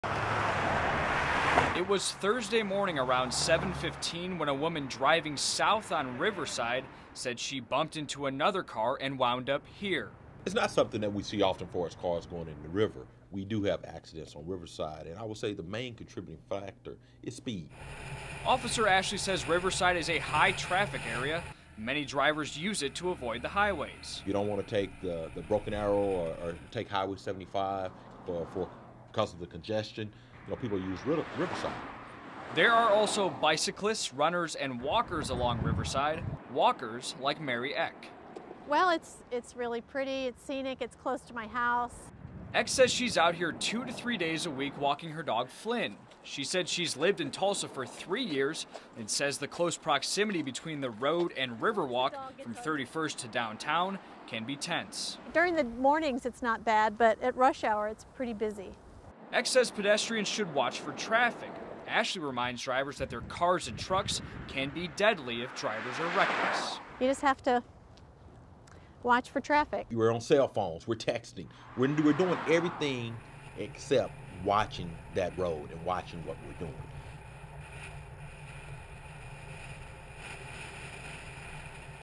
2 cars collide on Riverside, sending 1 vehicle into the Arkansas River
2 cars collide on Riverside, sending 1 vehicle into the Arkansas River